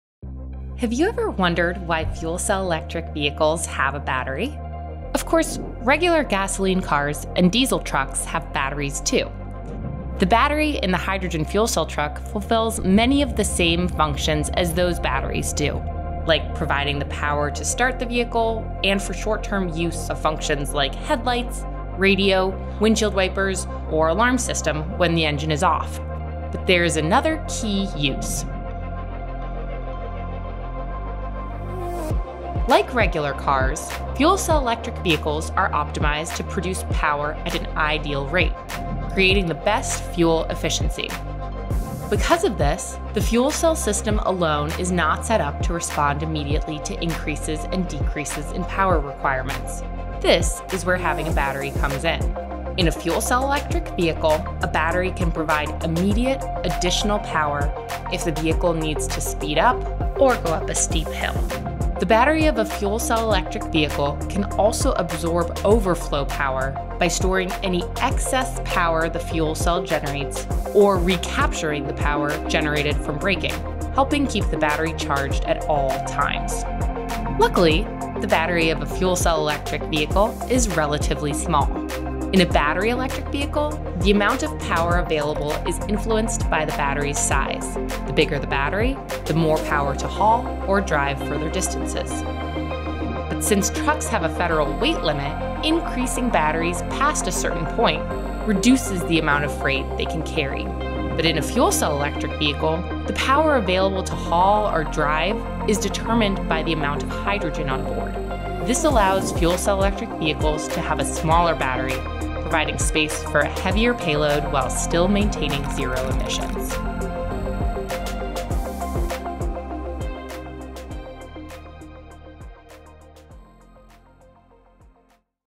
Why Do FCEVs Have a Battery? | Hyzon Motors Explains
Have you ever wondered why fuel cell electric vehicles (FCEVs) have a battery? In this video, Hyzon Motors breaks down the role that the battery plays in powering our eco-friendly trucks. From providing immediate additional power to storing excess energy generated from braking, learn how the battery helps maintain zero emissions while the fuel cell provides power to haul heavy loads. Watch now to discover the secret behind the battery in FCEVs!

💡 Hyzon Motors is bringing fuel cell vehicles to the heavy duty transportation sector.